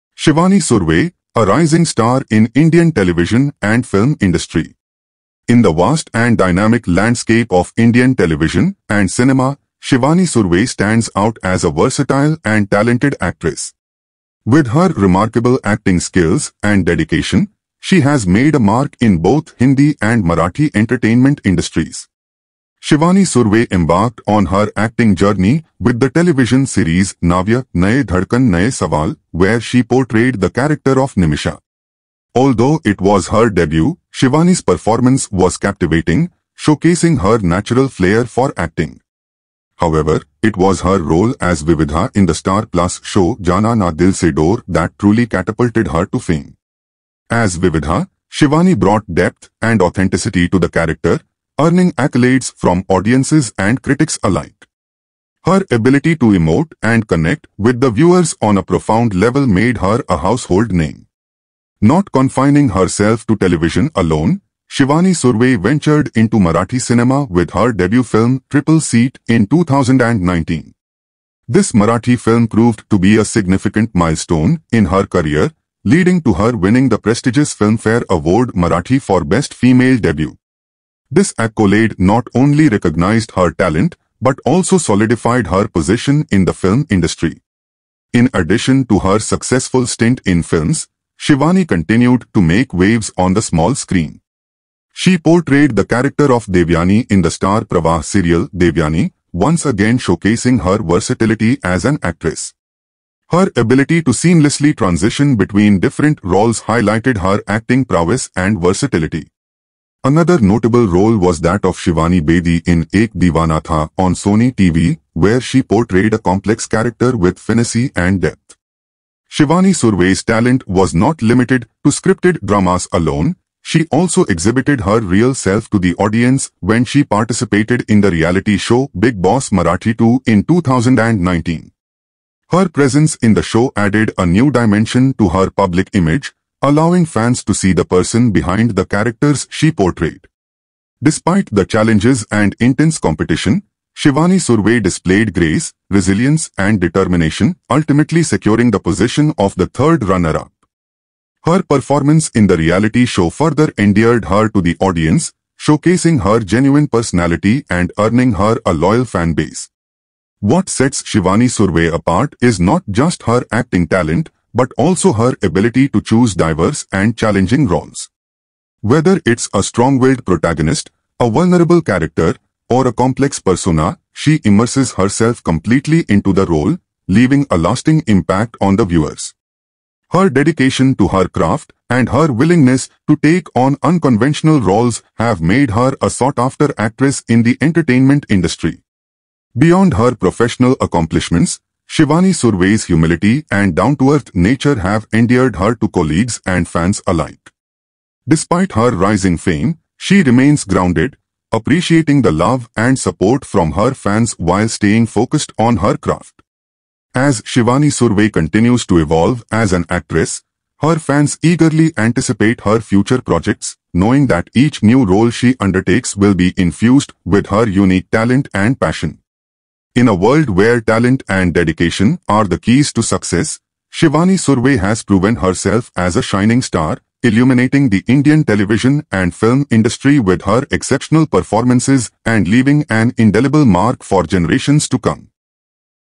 Shivani Surve biography
Shivani Surve: A Rising Star in Indian Television and Film Industry

In the vast and dynamic landscape of Indian television and cinema, Shivani Surve stands out as a versatile and talented actress. With her remarkable acting skills and dedication, she has made a mark in both Hindi and Marathi entertainment industries.

Shivani Surve embarked on her acting journey with the television series "Navya..Naye Dhadkan Naye Sawaal," where she portrayed the character of Nimisha. Although it was her debut, Shivani's performance was captivating, showcasing her natural flair for acting. However, it was her role as Vividha in the Star Plus show "Jaana Na Dil Se Door" that truly catapulted her to fame. As Vividha, Shivani brought depth and authenticity to the character, earning accolades from audiences and critics alike. Her ability to emote and connect with the viewers on a profound level made her a household name.

Not confining herself to television alone, Shivani Surve ventured into Marathi cinema with her debut film "Triple Seat" in 2019. This Marathi film proved to be a significant milestone in her career, leading to her winning the prestigious Filmfare Award Marathi for Best Female Debut. This accolade not only recognized her talent but also solidified her position in the film industry.

In addition to her successful stint in films, Shivani continued to make waves on the small screen. She portrayed the character of Devyani in the Star Pravah serial "Devyani," once again showcasing her versatility as an actress. Her ability to seamlessly transition between different roles highlighted her acting prowess and versatility. Another notable role was that of Shivani Bedi in "Ek Deewaana Tha" on Sony TV, where she portrayed a complex character with finesse and depth.

Shivani Surve's talent was not limited to scripted dramas alone; she also exhibited her real self to the audience when she participated in the reality show "Bigg Boss Marathi 2" in 2019. Her presence in the show added a new dimension to her public image, allowing fans to see the person behind the characters she portrayed. Despite the challenges and intense competition, Shivani Surve displayed grace, resilience, and determination, ultimately securing the position of the 3rd Runner up. Her performance in the reality show further endeared her to the audience, showcasing her genuine personality and earning her a loyal fan base.

What sets Shivani Surve apart is not just her acting talent but also her ability to choose diverse and challenging roles. Whether it's a strong-willed protagonist, a vulnerable character, or a complex persona, she immerses herself completely into the role, leaving a lasting impact on the viewers. Her dedication to her craft and her willingness to take on unconventional roles have made her a sought-after actress in the entertainment industry.

Beyond her professional accomplishments, Shivani Surve's humility and down-to-earth nature have endeared her to colleagues and fans alike. Despite her rising fame, she remains grounded, appreciating the love and support from her fans while staying focused on her craft.

As Shivani Surve continues to evolve as an actress, her fans eagerly anticipate her future projects, knowing that each new role she undertakes will be infused with her unique talent and passion. In a world where talent and dedication are the keys to success, Shivani Surve has proven herself as a shining star, illuminating the Indian television and film industry with her exceptional performances and leaving an indelible mark for generations to come.